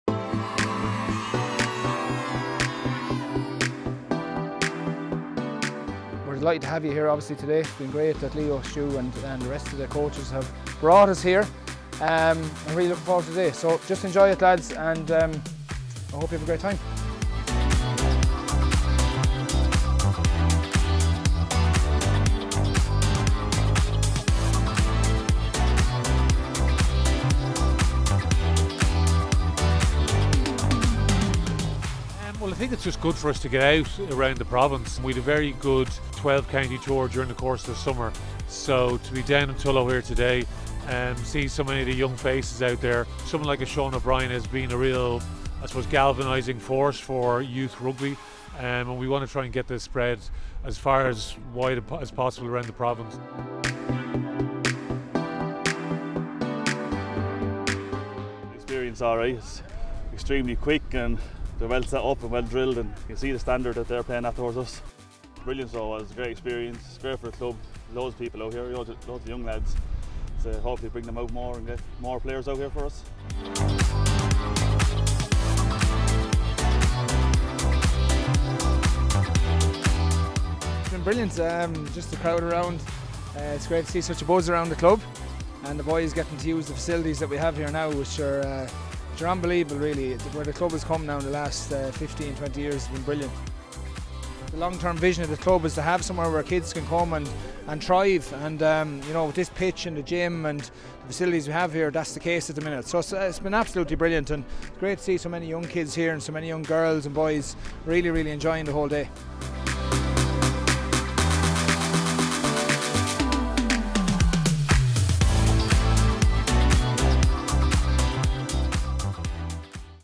Behind the scenes | Open training session at Tullow RFC
Behind the scenes at our open training session at Tullow RFC 🙌